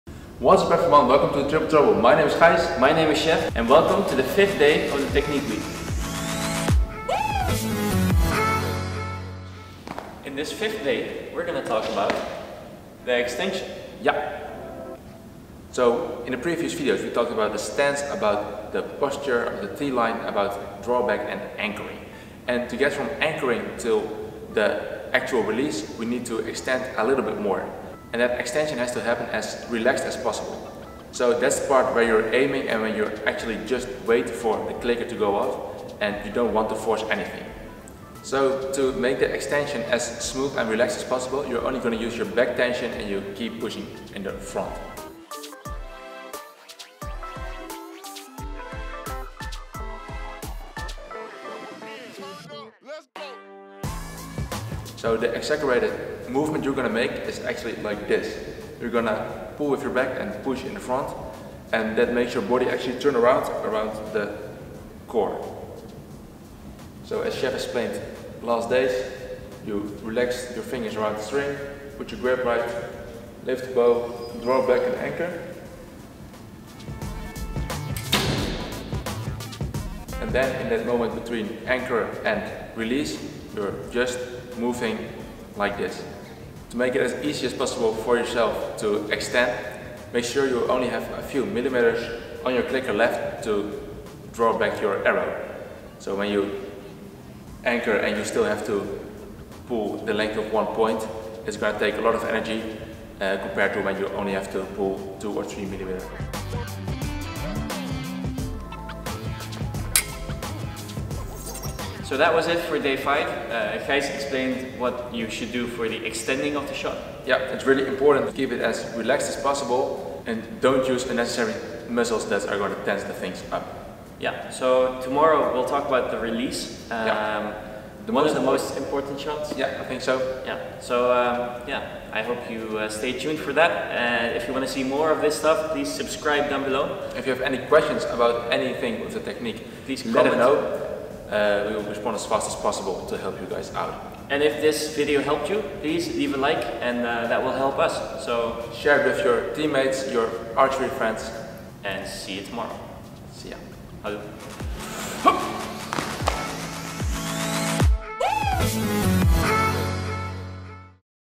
ARCHERY EXTENSION - TechniqueWeek #5
The extension can make a whole lot of difference in your shot. Archery as a sport is a game of precision, so in your technique you should be precise, too. By making sure you extend in the proper way you can be consistent and improve your results.
We hope you enjoy watching this video and learn something from it!